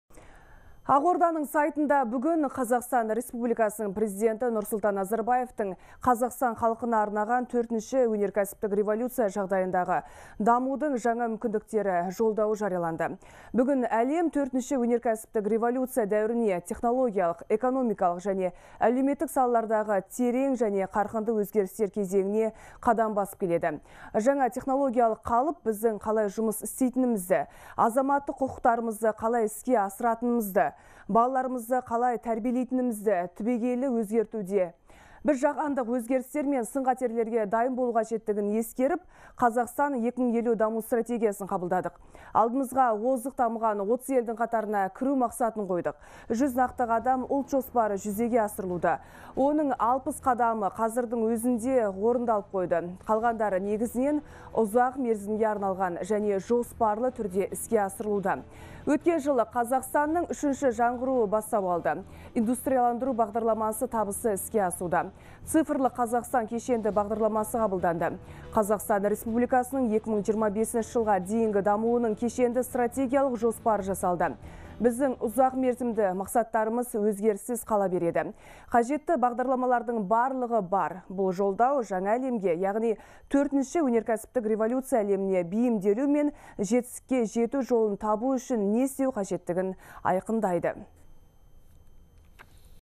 Елбасының Қазақстан халқына арнаған Жолдауы жарияланды
Республикалық «Егемен Қазақстан» газетінде бүгін Қазақстан Республикасының Президенті Нұрсұлтан Назарбаевтың Қазақстан халқына арнаған «Төртінші өнеркәсіптік революция жағдайындағы дамудың жаңа мүмкіндіктері» Жолдауы жарияланды.